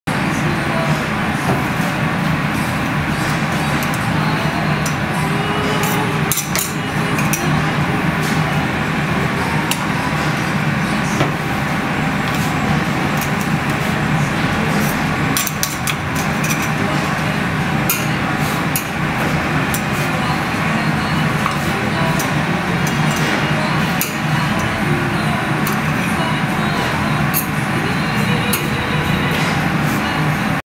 Termini Bros Pizzelles
This tradtional Anise Italian waffle cookie is still handmade on the original irons. Once cooled, they are cut into perfect circles. The pizzelle is one of our most beloved products